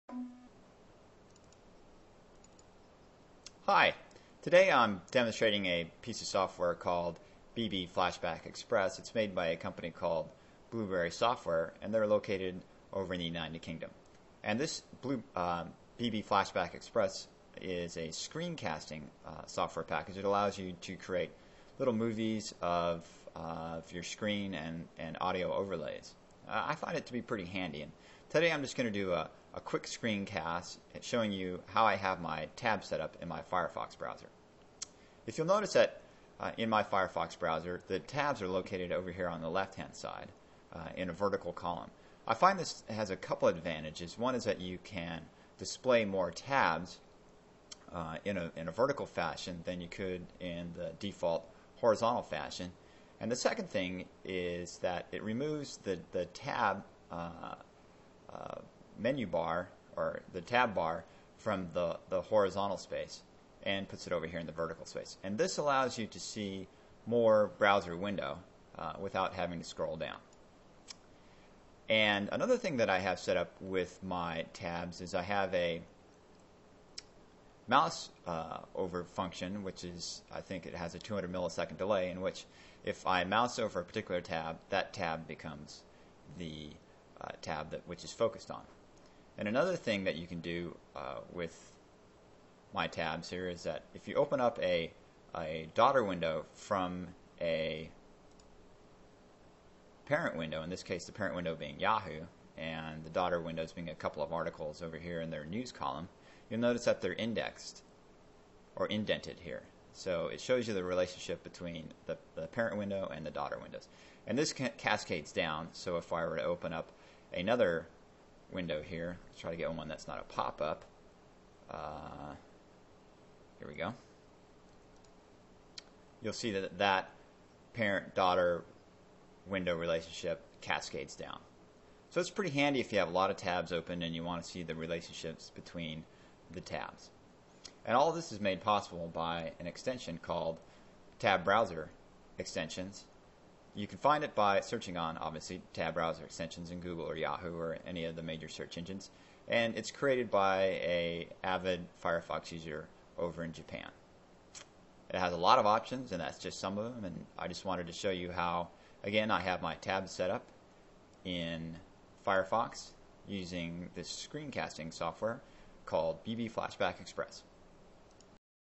Firefox Tab Setup - Test Screencast
Using Blueberry Software's BB Flashback Express I've created a test screencast showing the configuration of my Firefox tabs.